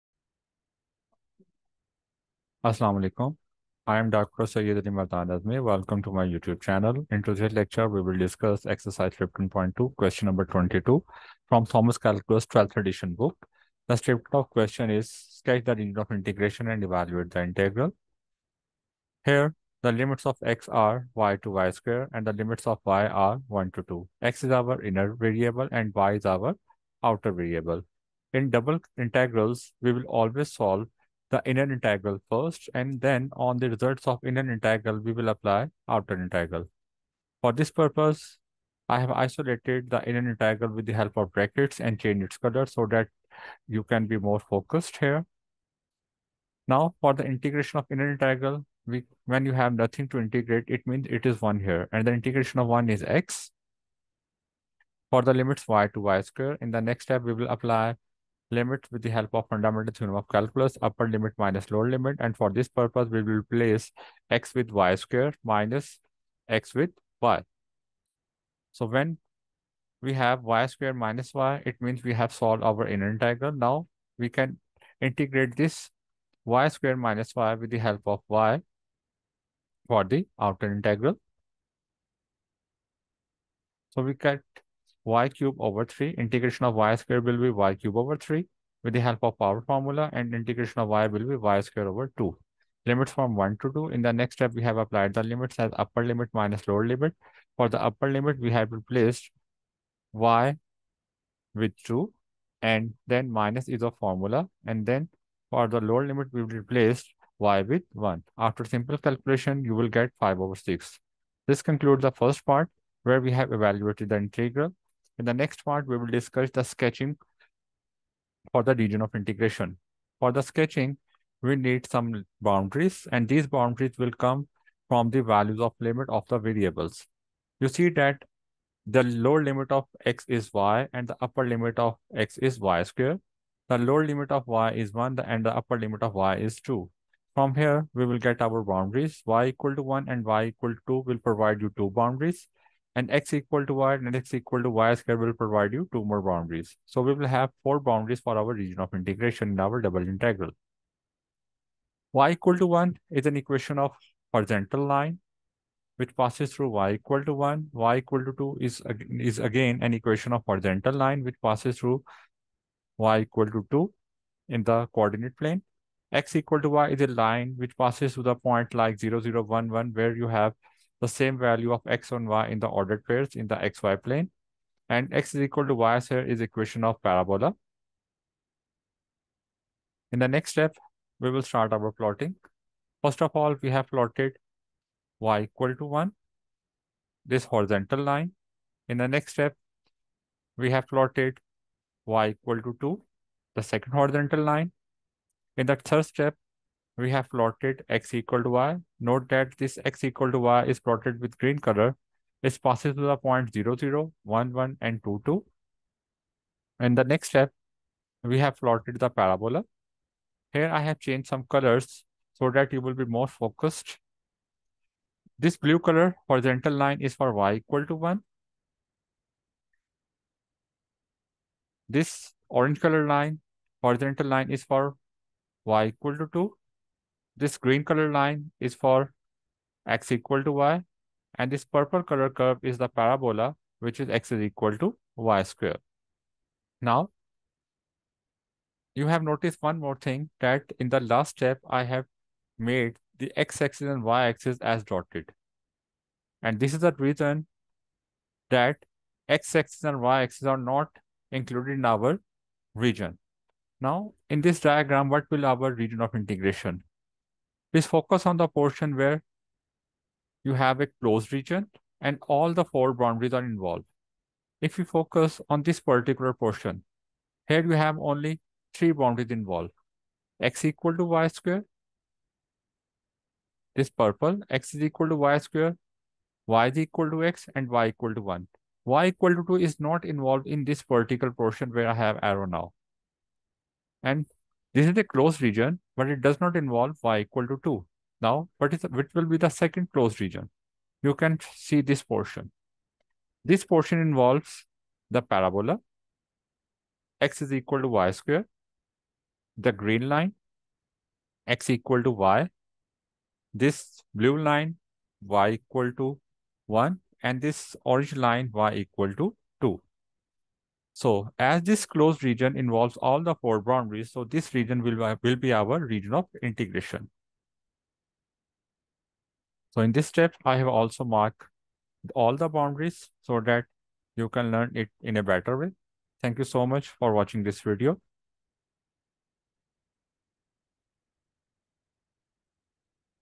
Ex 15.2 Q22 Thomas Calculus 12th Edition
Master Exercise 15.2, Question 22 in Thomas Calculus 12th Edition with our comprehensive step-by-step solution guide. Learn key calculus concepts, visualizations, and problem-solving strategies. Perfect for students and enthusiasts.